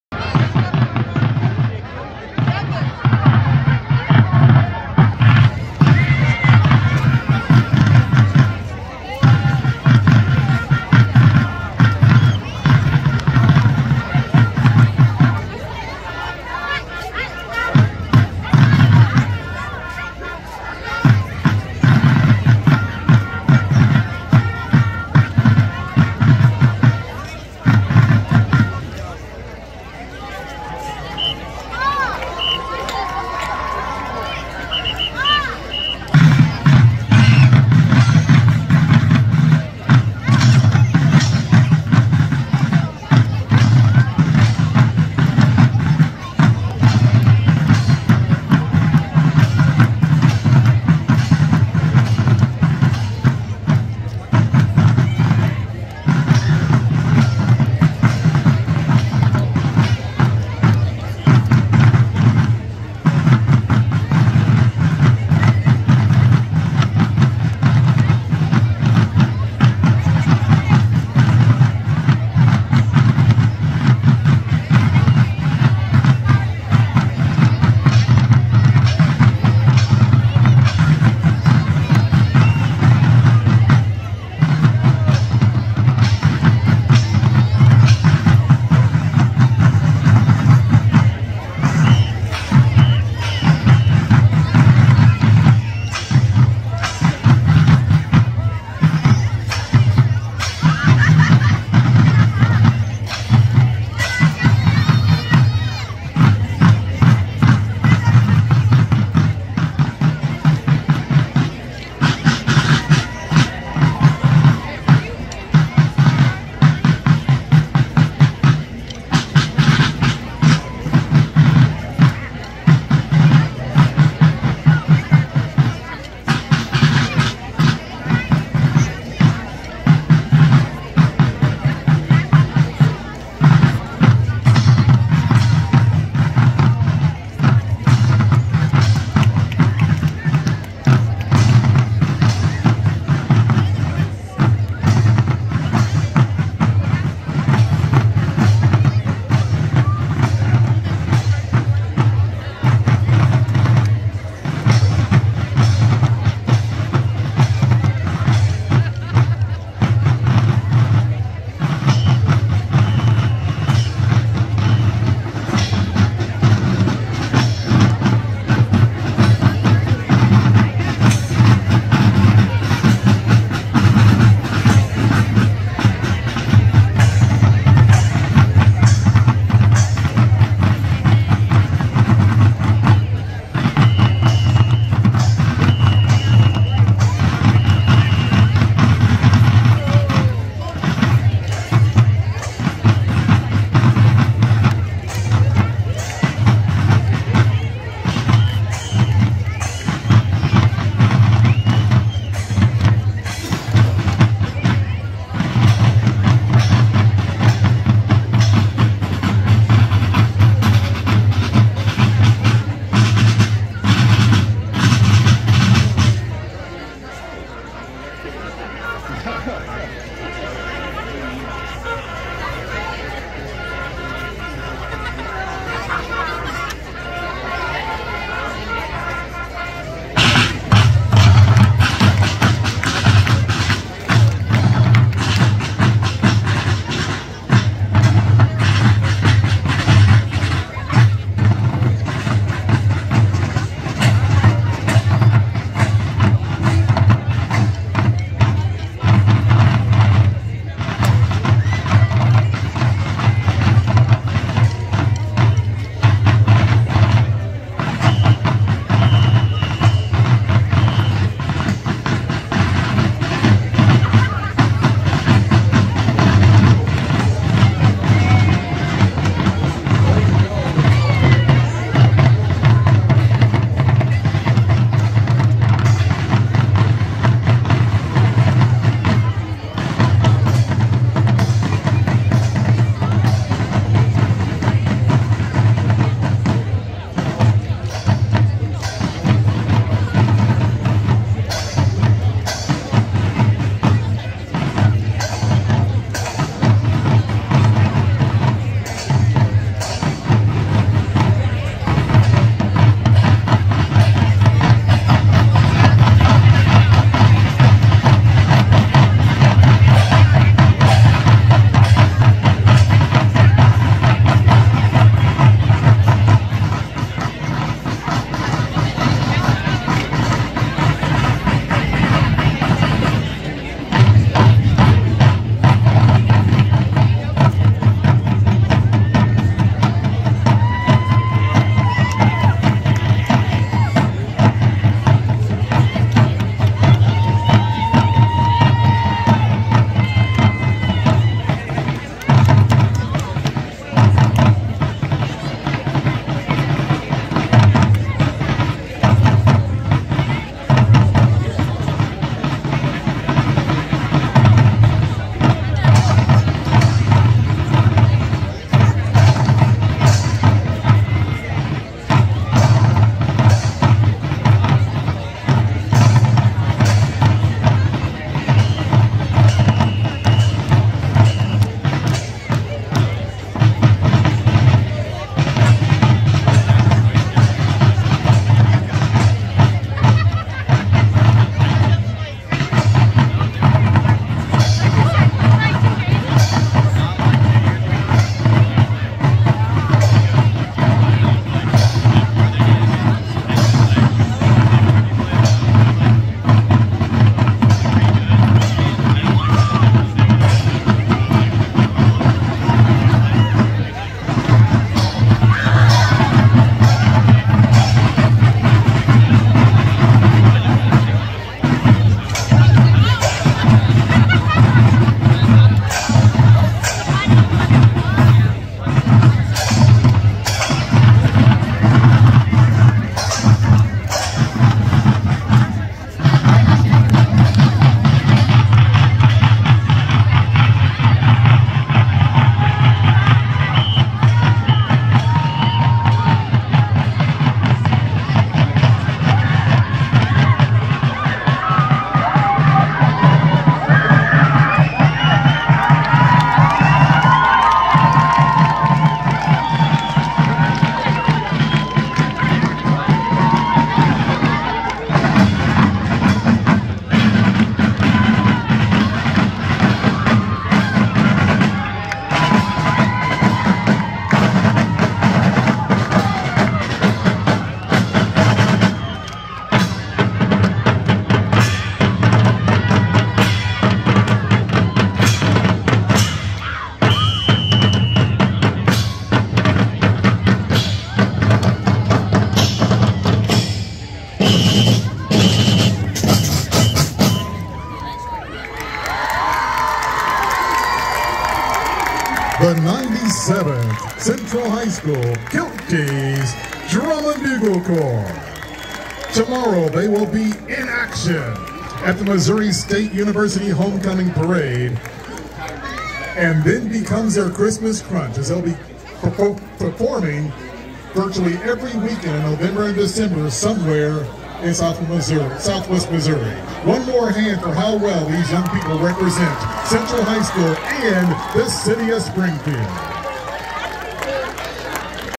Central High School Kilties - Halftime Performance - October 28, 2022
The 97th Kilties take the field for their last home field performance of the year at Central's biggest game in decades. (Apologies for the shaky video)